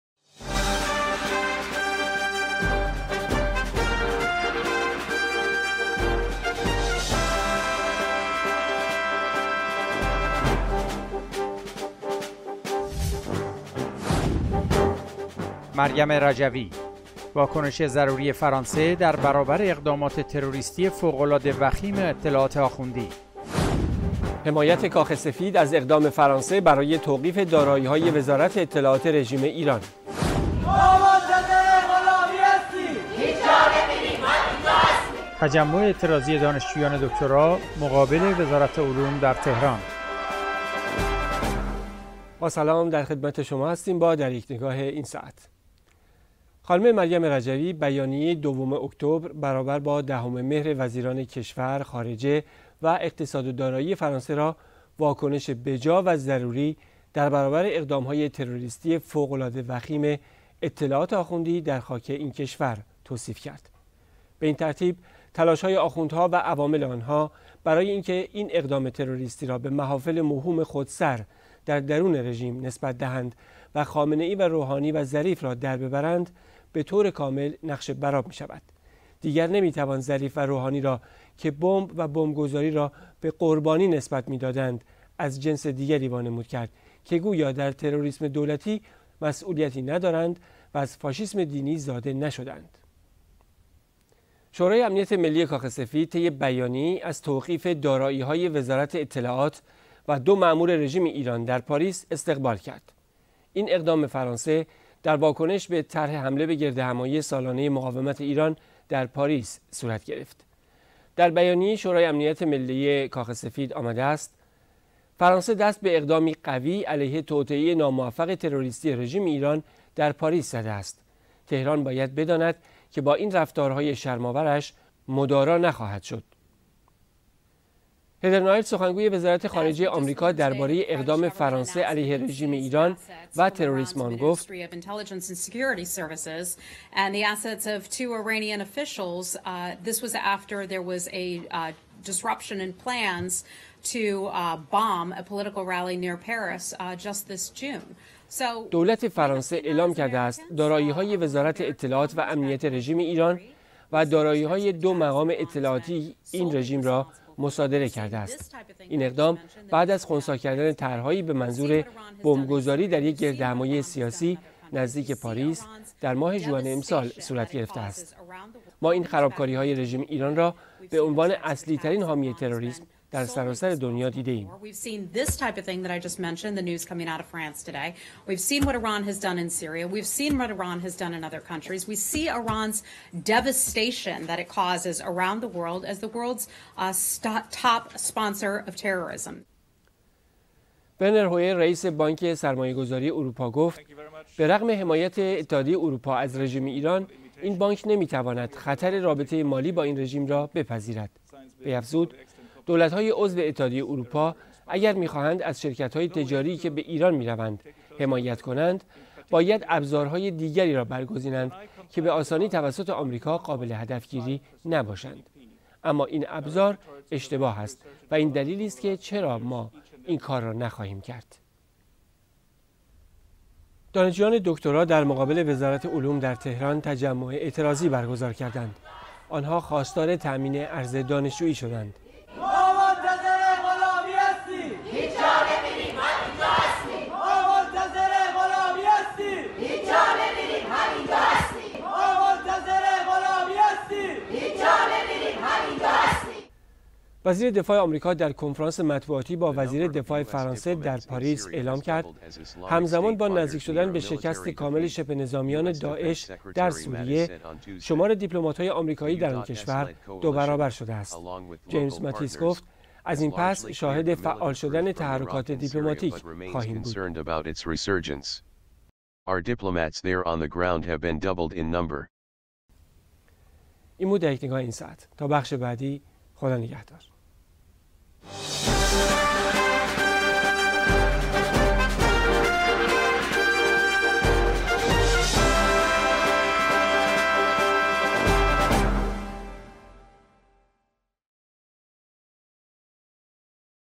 مریم رجوی، واكنش ضروری فرانسه در برابر اقدامات تروريستی اطلاعات ایران -در یک نگاه۱۱۳۰ -۹۷۰۷۱۱
1. ‌ مریم رجوی: واكنش ضروری فرانسه در برابر اقدامات تروريستی فوق العاده وخيم اطلاعات آخوندی
۲.حمایت کاخ سفید از اقدام فرانسه برای توقیف داراییهای وزارت اطلاعات رژیم ایران
۳.تجمع اعتراضی دانشجویان دکترا در  مقابل وزارت علوم در تهران(فیلم)

مریم رجوی: واكنش ضروری فرانسه در برابر اقدامات تروريستی فوق العاده وخيم اطلاعات آخوندی
خانم مريم رجوي، بيانيه دوم اكتبر (دهم مهر) وزیران كشور، خارجه و اقتصاد و دارايي فرانسه را واكنش به‌جا و ضروري در برابر اقدامهای تروريستي «فوق‌العاده وخيم» اطلاعات آخوندی در خاك اين كشور توصيف كرد.
به اين ترتيب تلاشهاي آخوندها و عوامل آنها براي اينكه اين اقدام تروريستي را به محافل موهوم خودسر در درون رژيم نسبت دهند و خامنه‌اي و روحاني و ظريف را در ببرند، به طور کامل نقش بر آب مي شود. ديگر نمي توان ظريف و روحاني را كه بمب و بمب گذاري را به قرباني نسبت مي دادند از جنس ديگري وانمود كرد كه گويا در تروريسم دولتي مسئوليتي ندارند و از فاشيسم ديني زاده نشده اند

حمایت کاخ سفید از اقدام فرانسه علیه تروریسم رژیم ایران
شورای امنیت ملی کاخ سفید طی بیانیه یی از توقیف دارایی‌های وزارت اطلاعات و دو مأمور رژیم ایران در پاریس استقبال کرد. این اقدام فرانسه  در واکنش به «طرح حمله به گردهمایی سالانه  مقاومت ایران» در پاریس صورت گرفت. در بیانیه شورای امنیت ملی کاخ سفید آمده است: «فرانسه دست به اقدامی قوی علیه توطئه ناموفق تروریستی رژیم ایران در پاریس زده است. تهران باید بداند که با این رفتارهای شرم‌آورش مدارا نخواهد شد»

وزارت خارجه آمریکا:‌ رژیم  ایران  منشأ تروريسم  است
هدر نائرت سخنگوی وزارت خارجه آمریکا درباره اقدام فرانسه  علیه رژیم ایران و تروریسم آن گفت: دولت فرانسه اعلام كرده است داراييهاي وزارت اطلاعات و امنيت  رژیم ايران و داراييهاي دو مقام اطلاعاتی  این رژیم را مصادره كرده است. اين اقدام  بعد از خنثي كردن طرحهايي به منظور بمبگذاري در يك گردهمايي سياسي  نزديك پاريس در ماه ژوئن همين سال صورت گرفته است.. ما اين خرابكاريهاي رژیم ايران را بعنوان اصلي ترين حامي تروريسم در سراسر دنيا   ديده ايم.

رئیس بانک سرمایه‌گذاری اروپا: نمی‌توانیم به خاطر رژیم ایران ریسک کنیم
ورنر هویر hoyer ، رئیس بانک سرمایه‌گذاری اروپا  گفت:‌ به ‌رغم حمایت اتحادیه اروپا  از رژیم ایران این بانک نمی تواند خطر رابطه مالی با  این رژیم  را بپذیرد. وی افزود: «دولت‌های عضو اتحادیه اروپا اگر می‌خواهند از شرکت‌های تجاری که به ایران می‌روند حمایت کنند، باید ابزارهای دیگری را برگزینند که به آسانی توسط آمریکا قابل هدف‌گیری نباشند. اما این ابزار اشتباه است و این دلیلی است که چرا ما این کار را نخواهیم کرد.

تهران:‌تجمع اعتراضی دانشجویان دکترا
دانشجویان دکترا در مقابل وزارت علوم  در تهران  تجمع اعتراضی برگزار  کردند. آنها خواستار تأمین ارز دانشجویی شدند.
ما منتظر غلامی هستیم هیچ جا نمیریم همینجا هستیم

جیمز متیس: شمار دیپلومات‌های آمریکایی در سوریه دو برابر شده است
وزیر دفاع آمریکا در کنفرانس مطبوعاتی با وزیر دفاع فرانسه در پاریس اعلام کرد همزمان با نزدیک شدن به شکست کامل شبه نظامیان داعش در سوریه، شمار دیپلمات‌های آمریکایی در آن کشور دو برابر شده است. جیمز متیس   گفت: از این پس شاهد فعال شدن تحرکات دیپلوماتیک خواهیم بود.